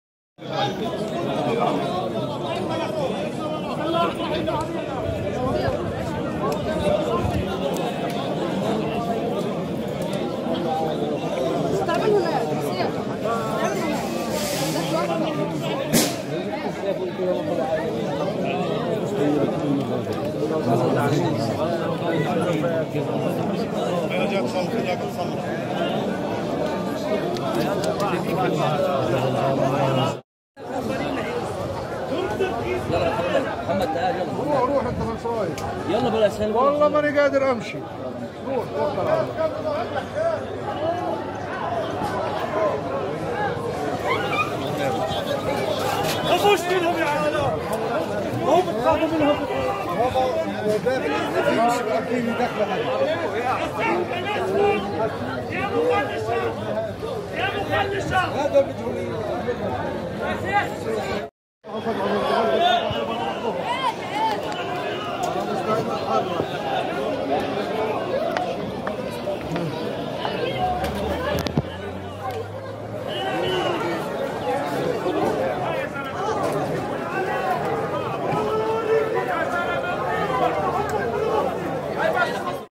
تجمع للمصلين عند باب الأسباط بعد منعهم من دخول المسجد الأقصى والقوات تعتدي عليهم بالدفع
تجمع للمصلين عند باب الأسباط بعد منعهم من دخول المسجد الأقصى والقوات تعتدي عليهم بالدفع رغم وجود أظفال ونساء وكبار بالسن في المكان